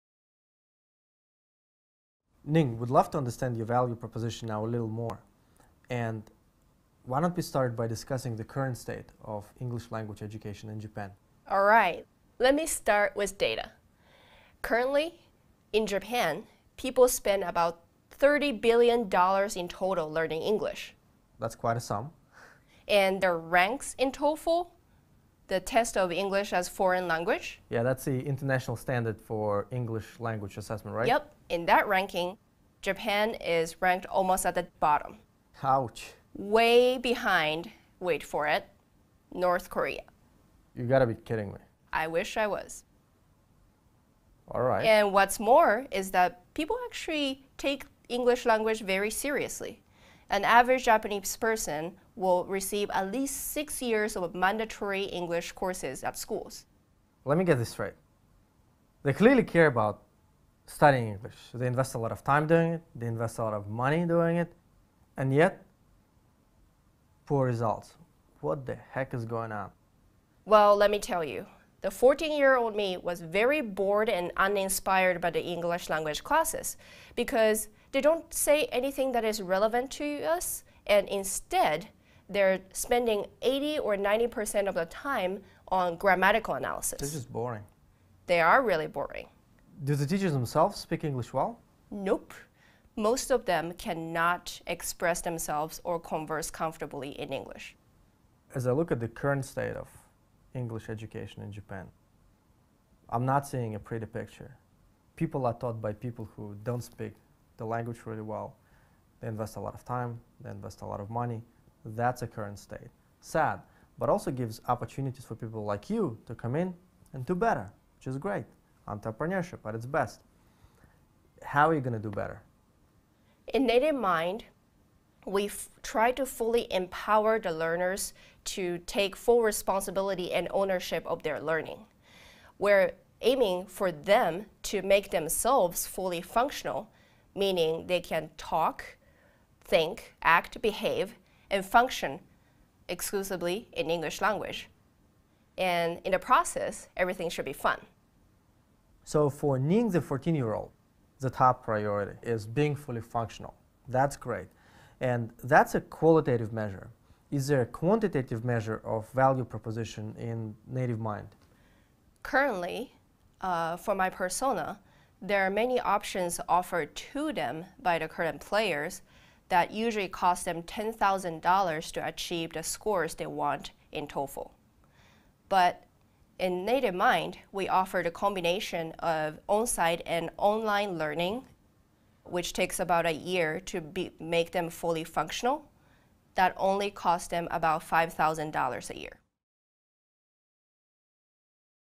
How to Quantify the Value Proposition: Native Mind Case Study
Ning Shirakawa, CEO of Native Mind, explains her company's Quantified Value Proposition, and how she arrived at it.

This video is part of MIT's Online Entrepreneurship Course ► ENTREPRENEURSHIP 102: WHAT CAN YOU DO FOR YOUR CUSTOMER?